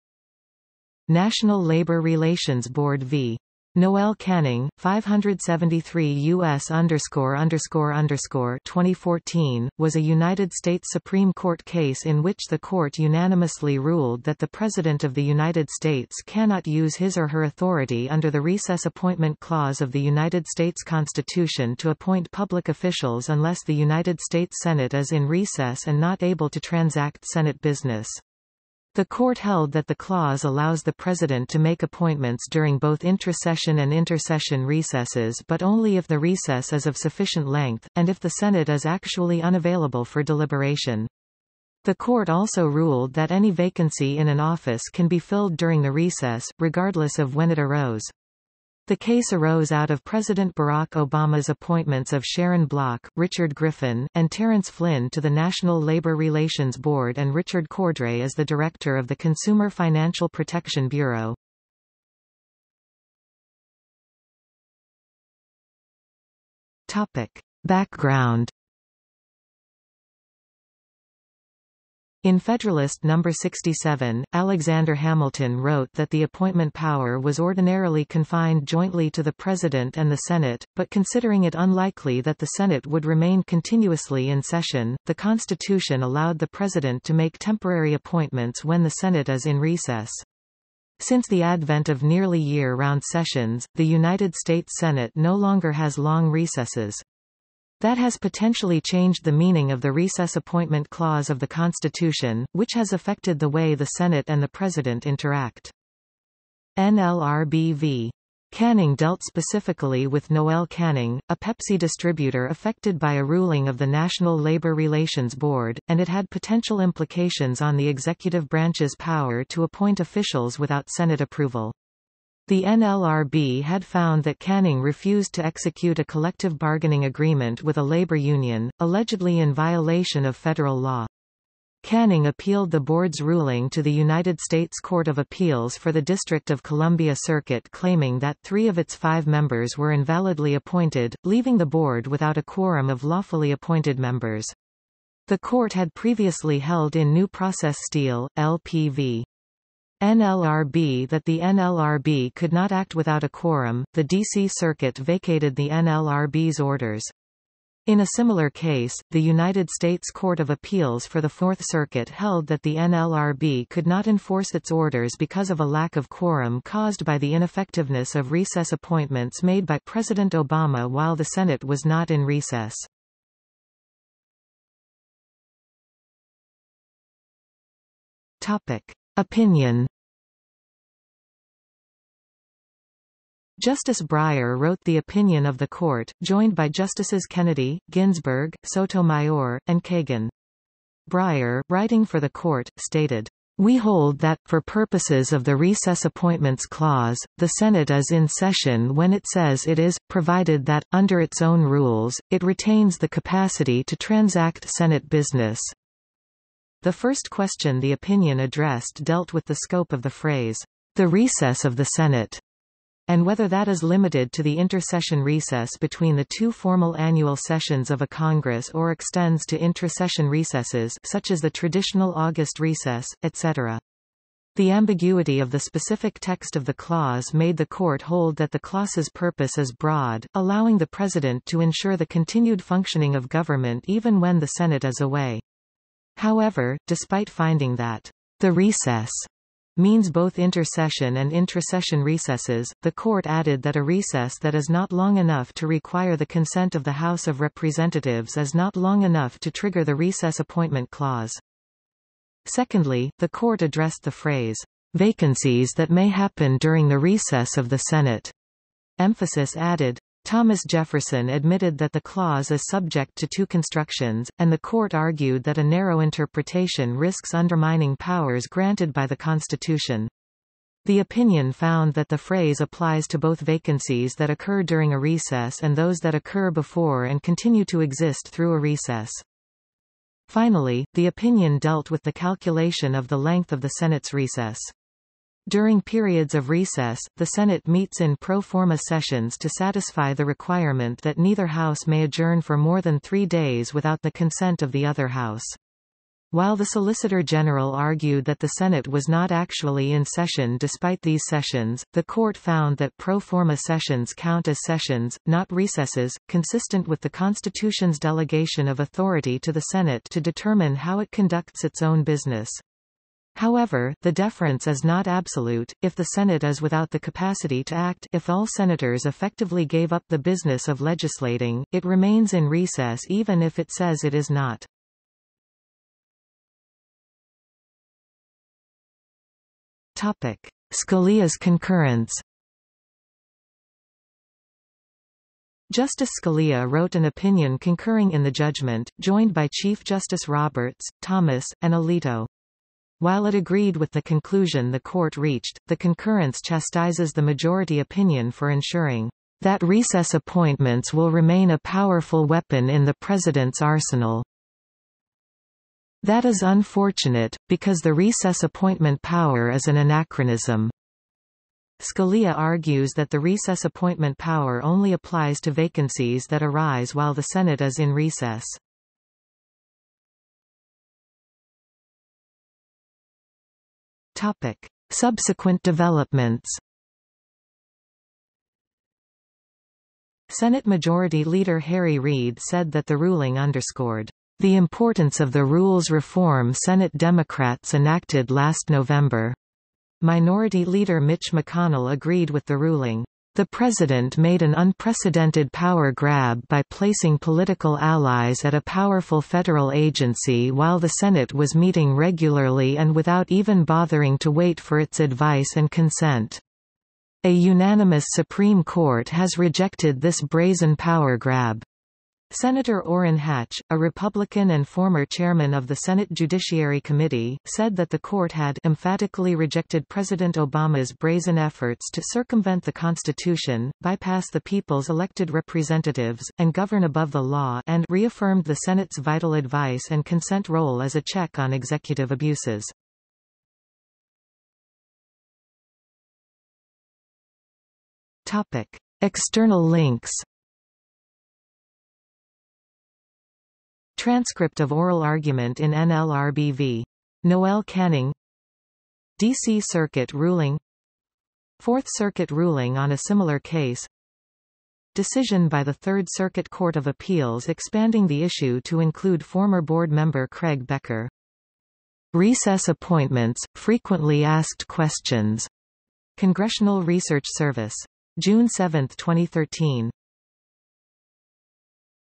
National Labor Relations Board v. Noel Canning | Wikipedia audio article
00:01:03 1 Background
00:02:54 2 Opinion
00:05:26 2.1 Scalia's concurrence
00:06:10 3 Subsequent developments
00:07:14 4 External links

- Socrates

SUMMARY
National Labor Relations Board v. Noel Canning, 573 U.S. ___ (2014), was a United States Supreme Court case in which the Court unanimously ruled that the President of the United States cannot use his or her authority under the Recess Appointment Clause of the United States Constitution to appoint public officials unless the United States Senate is in recess and not able to transact Senate business. The Court held that the clause allows the president to make appointments during both intra-session and inter-session recesses but only if the recess is of sufficient length, and if the Senate is actually unavailable for deliberation. The Court also ruled that any vacancy in an office can be filled during the recess, regardless of when it arose. The case arose out of President Barack Obama's appointments of Sharon Block, Richard Griffin, and Terence Flynn to the National Labor Relations Board and Richard Cordray as the director of the Consumer Financial Protection Bureau.